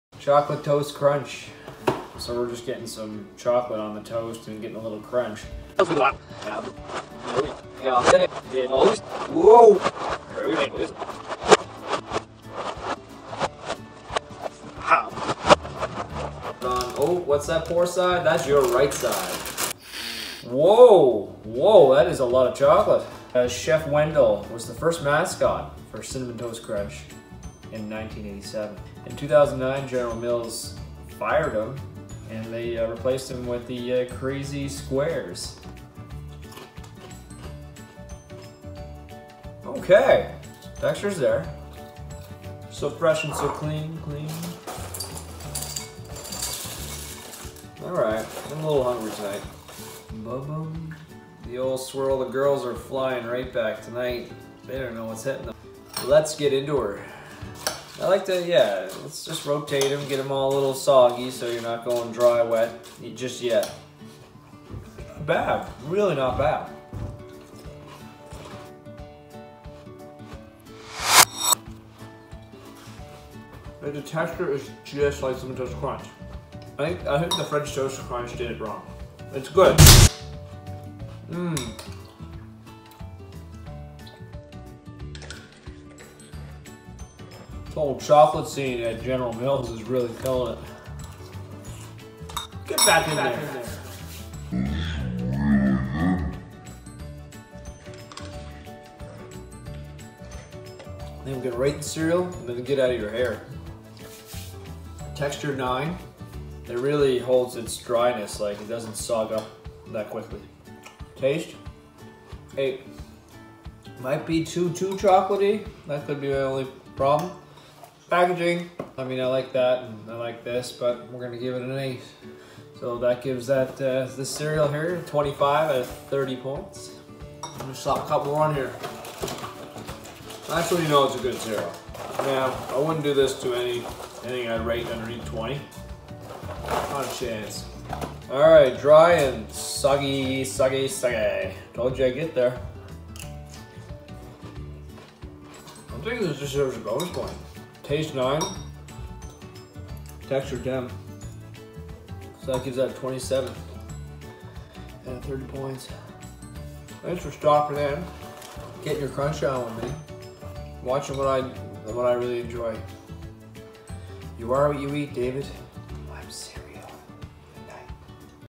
Chocolate Toast Crunch Review
Did @generalmills nail a good twist on an original??? Watch and see!